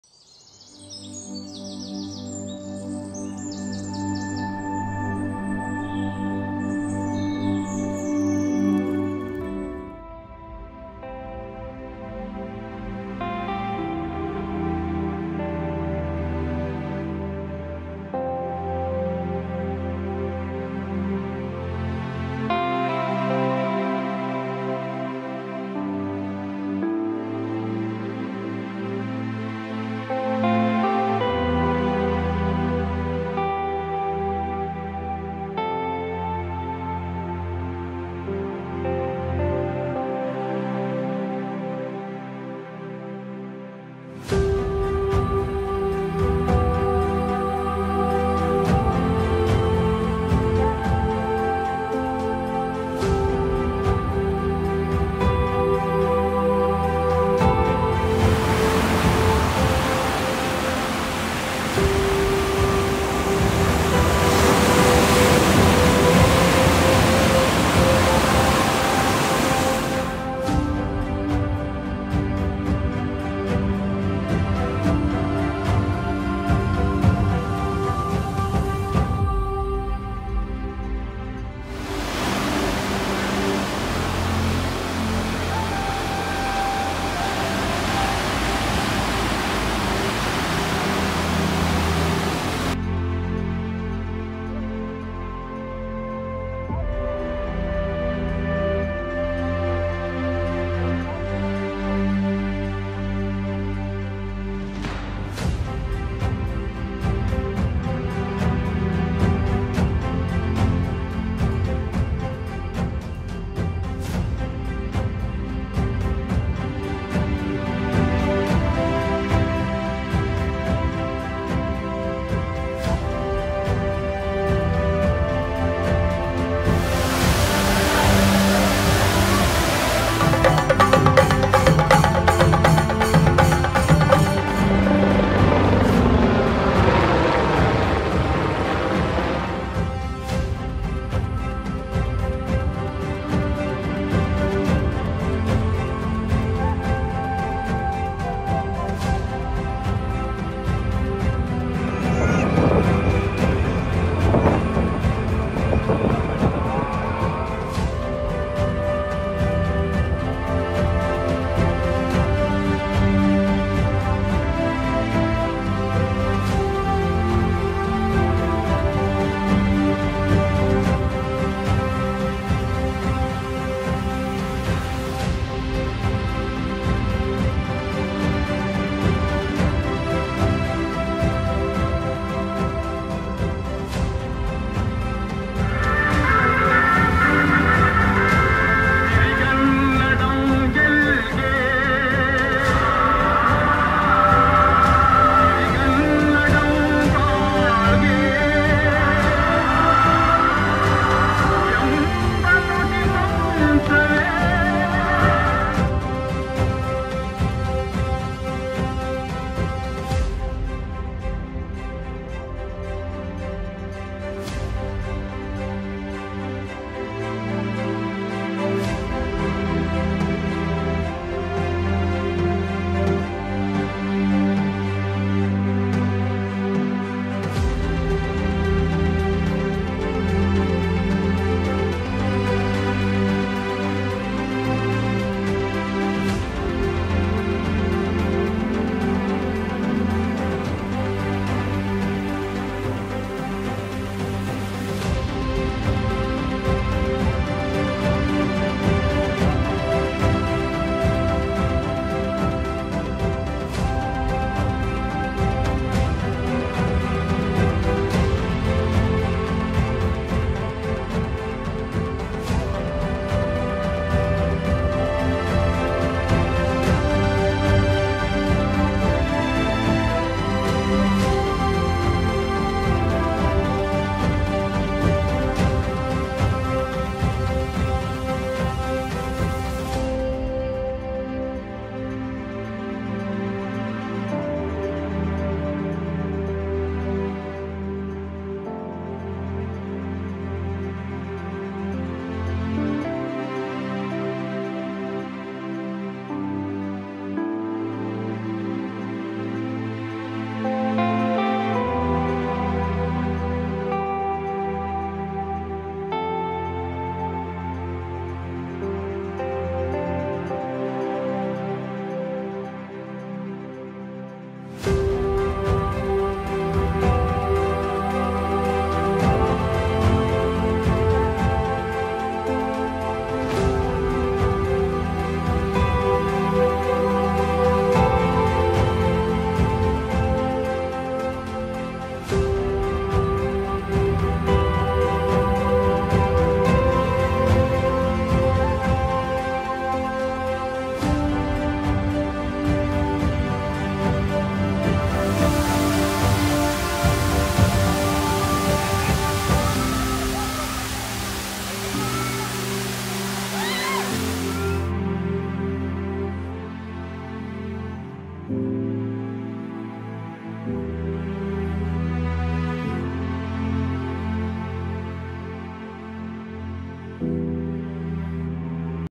iPhone 14 Pro Max Cinematic Mode
featuring an all-new 48mp Main camera with up to 4x resolution , action mode for smooth handheld action scenes, 4k cinematic mode, advanced low-light performance , and four zoom options to give you even more framing flexibility .